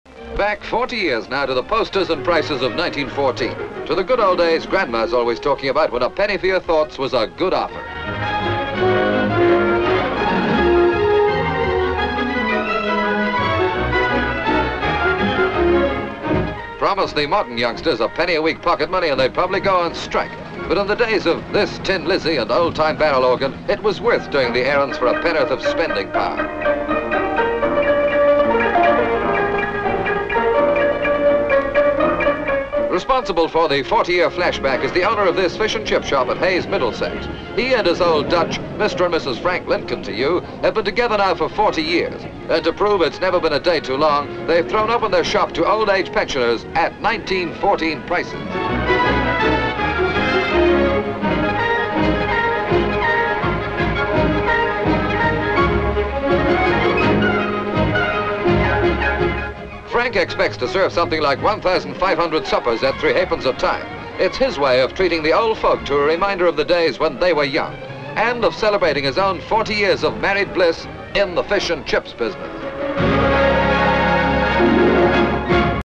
Fish And Chips (1954)
Mr Frank Lincoln serves fish and chips at 1914 prices to local pensioners to celebrate his 40th wedding anniversary.

Hayes, Middlesex.

C/U Kitchener poster "Your Country Needs You" from the First World War.  Various shots of various food, fruit and sweets labelled at 1914 prices.  M/S as a group of young boys run up to the shop window and point at the food and prices.  M/S as vintage car rolls up and stops, an elderly lady waves from it. M/S man playing barrel organ in the street, M/S monkey sitting on top.  M/S as pensioners walk into the fish and chip shop.  Various shots as the staff serve the customers in the packed shop.  M/S as woman puts flower in pensioner's coat lapel. M/S as woman in wheelchair wheels up with her plate and is handed the parcel over the counter.  M/S as man fills bags with chips. C/U handing over chips and taking money. C/U cash register.  M/S coins on counter.  C/U fish and chips being put into bag.  M/S people crowded outside his shop "Lincoln and Sons Ltd".
 FILM ID:1607.07